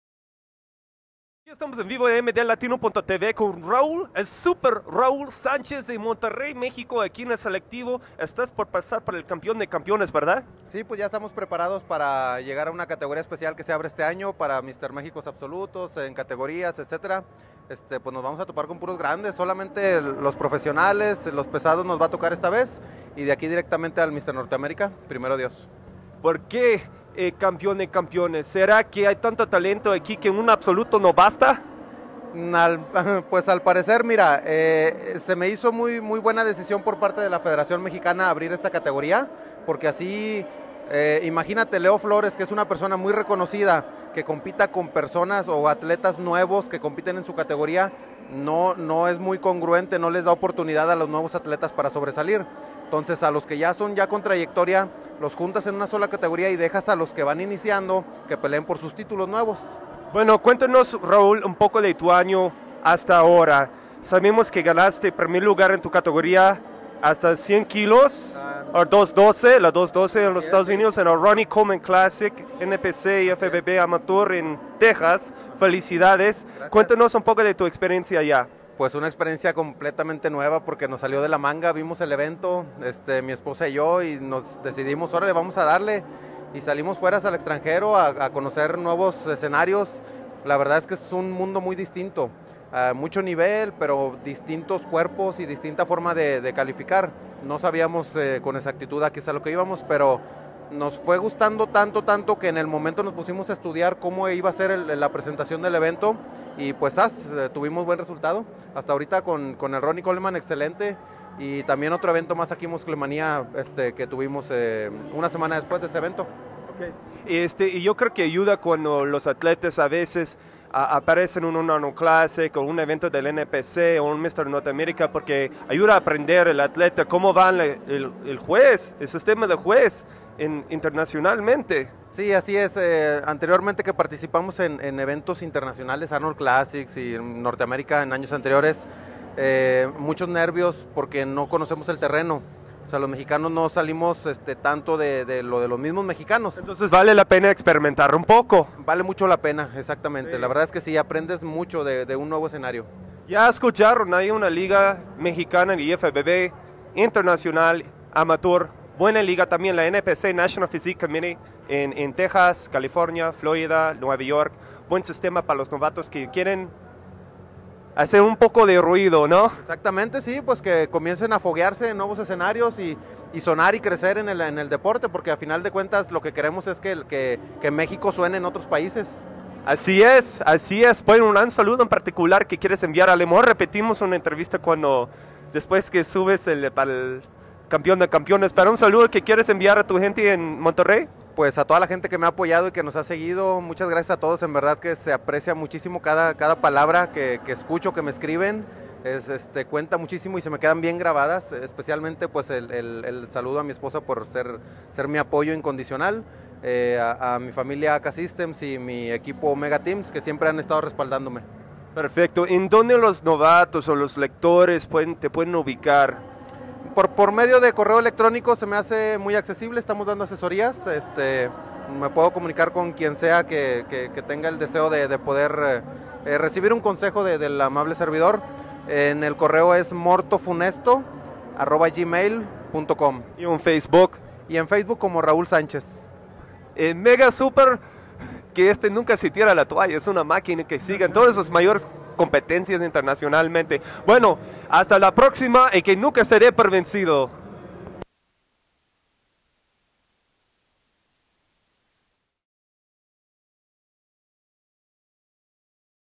RAUL SANCHEZ MD LATINO TV
KIT SANDERSON DE MD LATINO TV PRESENTA A RAUL SANCHEZ APENAS ANTES QUE RAUL SUBIA A LA TARIMA EN EL SELECTIVO NACIONAL DE MEXICO 2013. RAUL, UNO DE LOS MAS TALENTOSO DE MAS DE 100 KILOS EN MEXICO AHORA, NOS CUENTA SU EXPERINCIA EN LOS TORNEOS INTERNACIONALES Y LA IMPORTANCIA APRENDER DE ELLOS. BUSCA RAUL EN EL MR. MEXICO INTERNACIONAL, ENTRE OTROS TORNEOS INTERNACIONALES EN 2013 Y 2014.